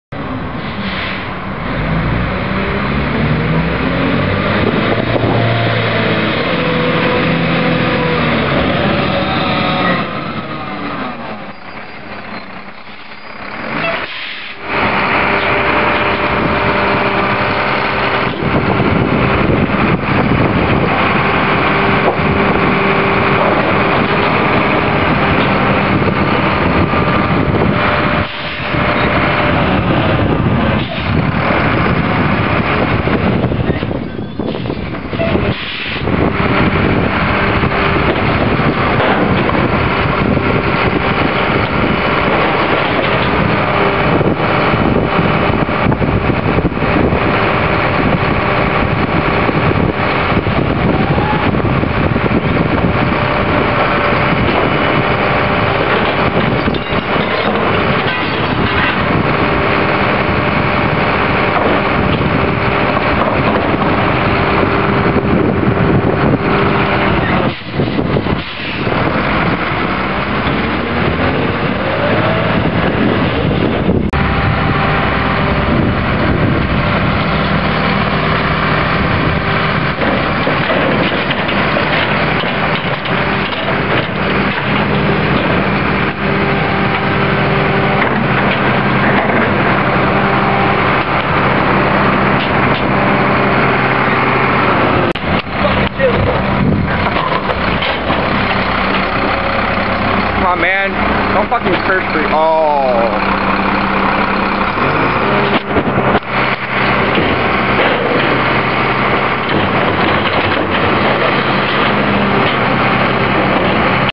GSS Volvo/McNeilus MSL
Caught up with my old friend John, you may remember him from Bill's (Shadofax96) video of Arrow Disposal Lodal posted a about six months ago. Well John works for Gresham Sanitary Services now and this was the truck he happened to be driving the day I was up in Portland for the holidays. It was cold as piss outside (reason for the camera shaking lol) and so I didn't get much footage. Hope you all enjoy what I got.